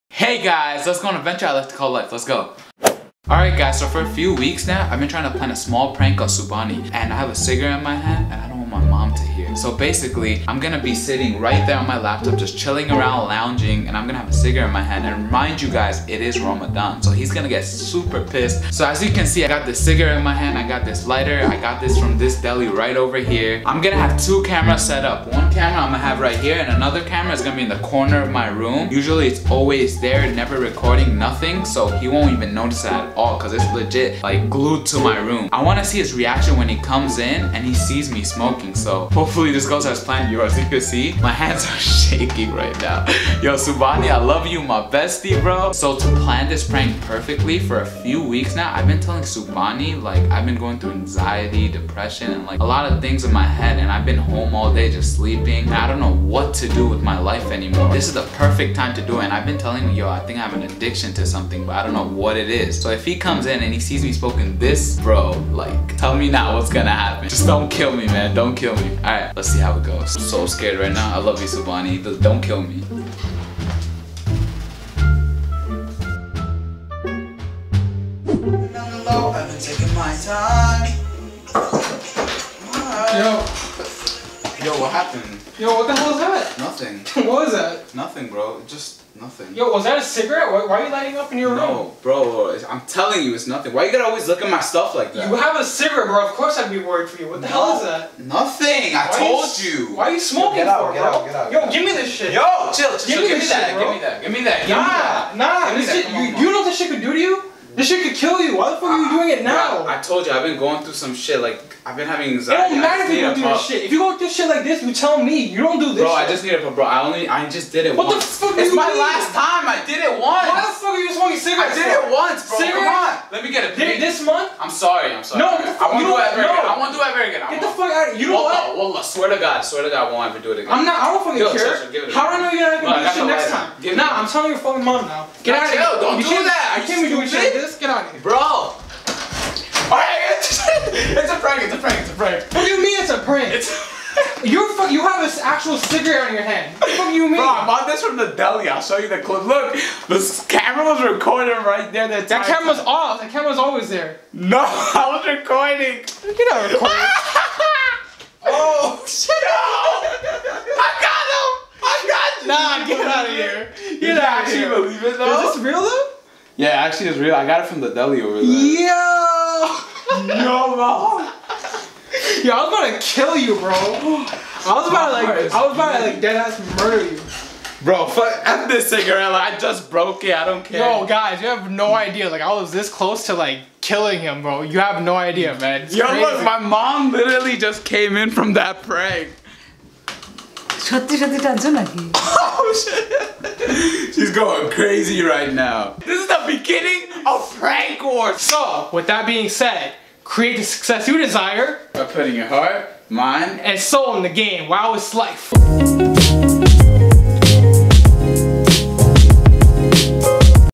SMOKING A CIGARETTE!!! *RAMADAN PRANK*
I decided to prank Subhani into thinking I am smoking a cigarette! This is the beginning of prank wars between MUQIT vs SUBHANI!!! Get ready. Love you guys.

(Subhani: @SUBEEGOO)
(Muqit: @M.TASFIC)

(Muqit: @MUQITTASFICWOW)
(Subhani: SUBEEGOO)